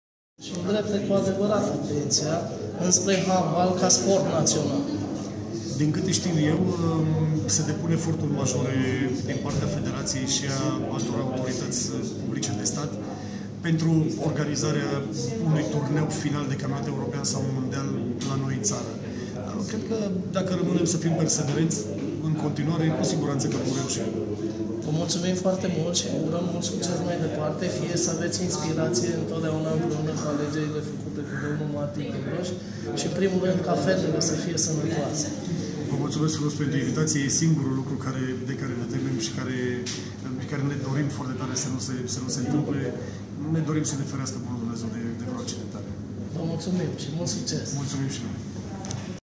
VIDEO - Antrenorul secund Horațiu Pașca după 19-17 România-Spania
la Campionatul Mondial de Handbal Germania 2017 exclusiv -partea 2